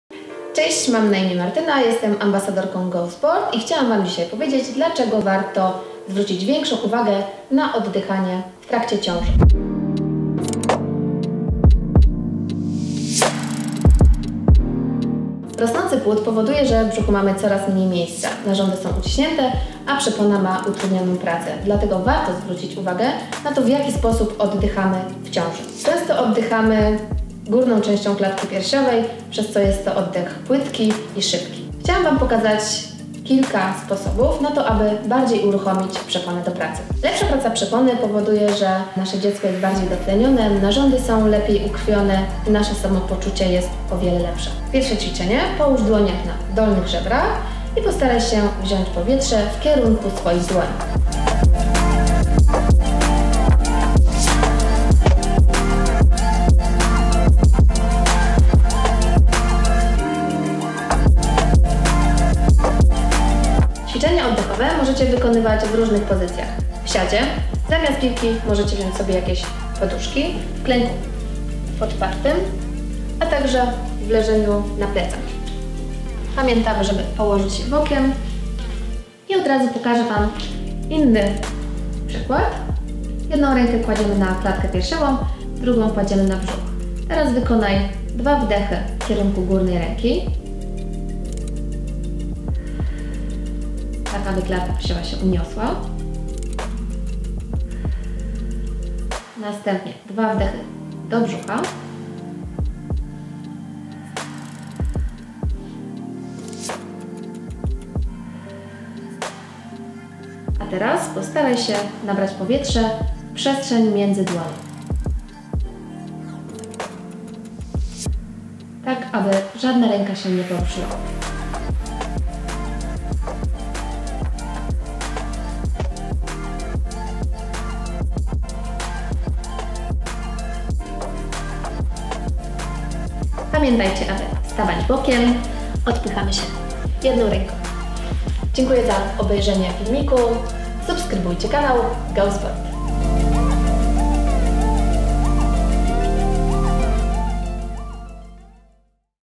Ćwiczenia w ciąży: oddychanie. Po co i jak ćwiczyć oddech? Radzi AmbasadorGOSport Martyna Langowska
Spodziewasz się dziecka? Czujesz, jak nowy człowiek rośnie w Twoim ciele, zabierając Ci z tygodnia na tydzień nie tylko coraz więcej sił, lecz także spłycając Twój oddech? AmbasadorGOSport Martyna Langowska podpowie Ci, jak sobie z tym poradzić!

Kiedy jesteś w ciąży, dostarczasz swojemu dziecku nie tylko pokarmu i składników odżywczych, dzięki którym się rozwija. Dziecko przede wszystkim oddycha tym, co Ty – tlen jest transportowany przez krwiobieg matki. Dlatego tym bardziej zwracaj uwagę na swój sposób oddychania.

Obejrzyj film, który przygotowała AmbasadorGOSport, Martyna Langowska, która na co dzień zajmuje się pomocą ciężarnym i młodym mamom w dbaniu o formę, ciało i zdrowie. Rozmowę z Martyną, w której mówi o tym, czym się zajmuje i dlaczego zdecydowała się właśnie na taką pracę, przeczytasz na GO Blogu.

Z tego filmu dowiesz się:
- jak prawidłowo oddychać w ciąży
- jakie ćwiczenia wykonywać, by ułatwić sobie oddychanie

Możesz także obejrzeć inne filmy GO Sport na naszym kanale YouTube:
1. Jak ćwiczyć jogę?